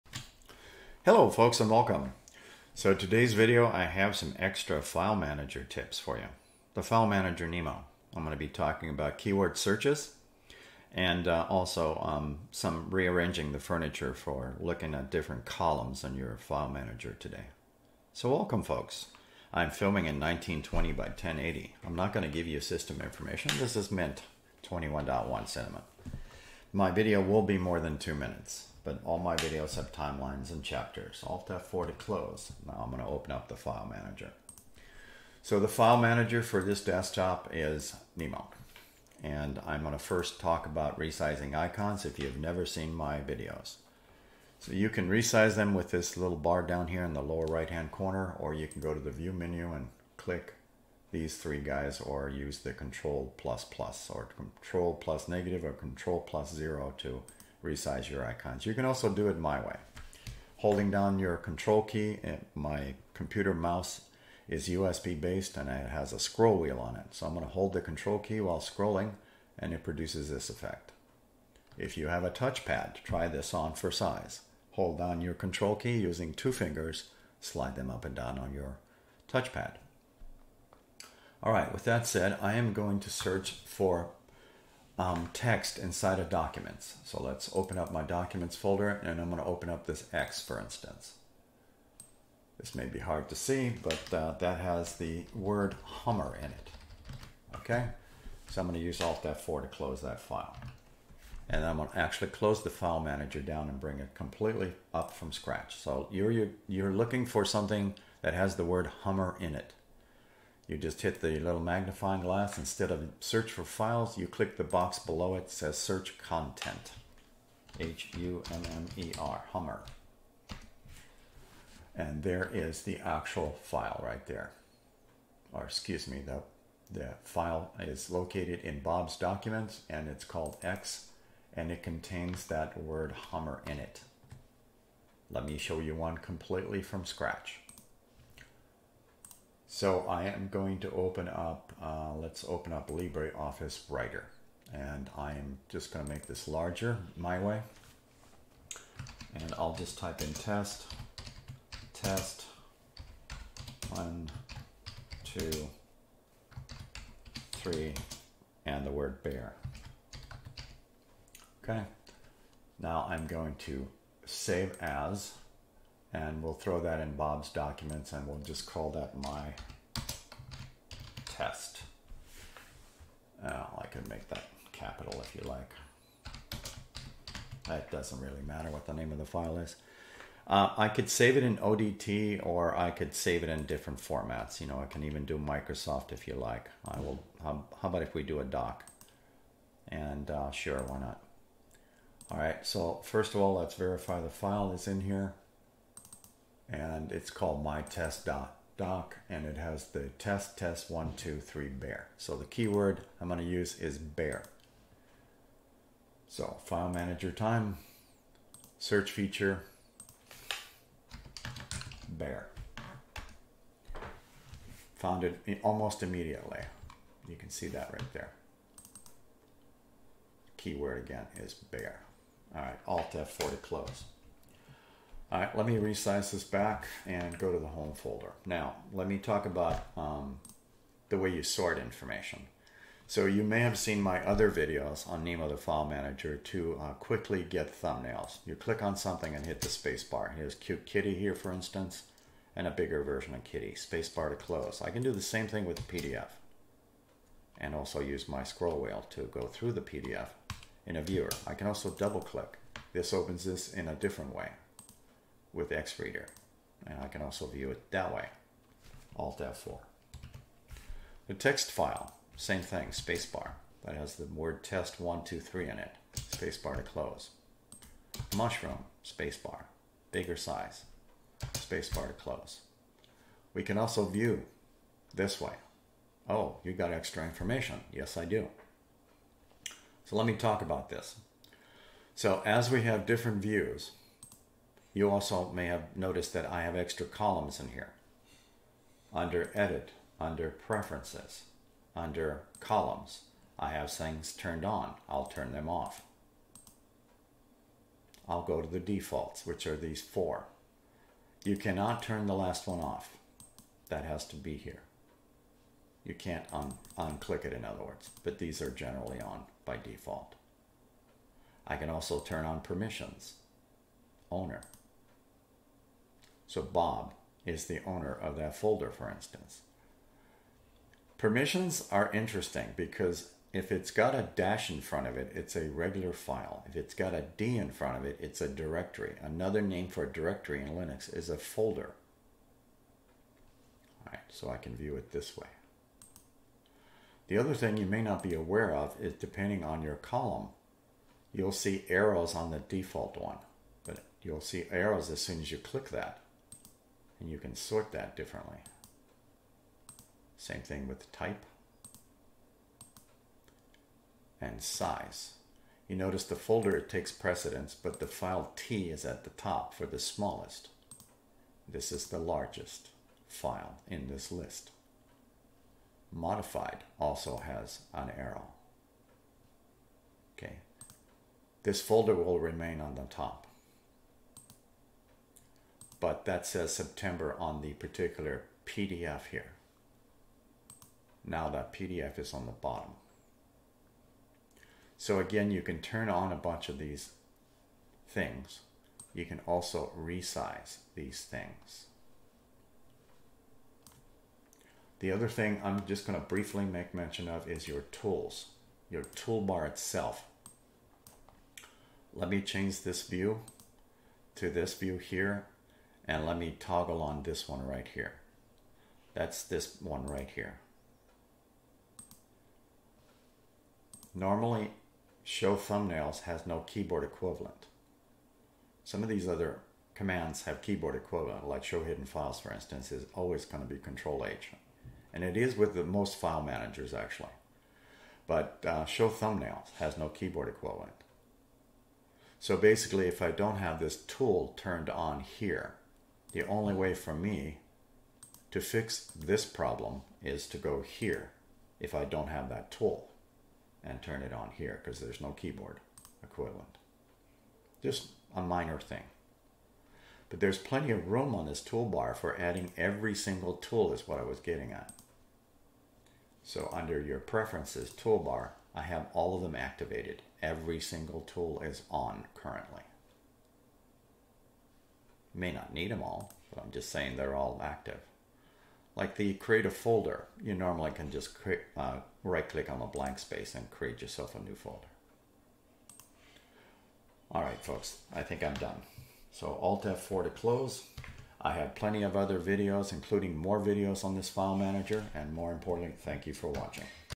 Linux Mint 21.1 - Cinnamon - Nemo File Manager Extra Search Tips.
Filmed in 1920 x 1080. Extra file manager Nemo Search content tips and extra columns tips. Best viewed on large screen.

0:00 - Intro Linux mint 21.1 Search Tips & Columns in Nemo File Manager
0:08 - Overview
1:25 - Text Search inside of file on Nemo File Manager
3:52 - Examples of viewer with using keyboard space bar
4:35 - Example of view columns
4:50 - How to add extra Columns in file manager Nemo
6:38 - Nemo Toolbar tips
7:58 - Subscribe to Linux for seniors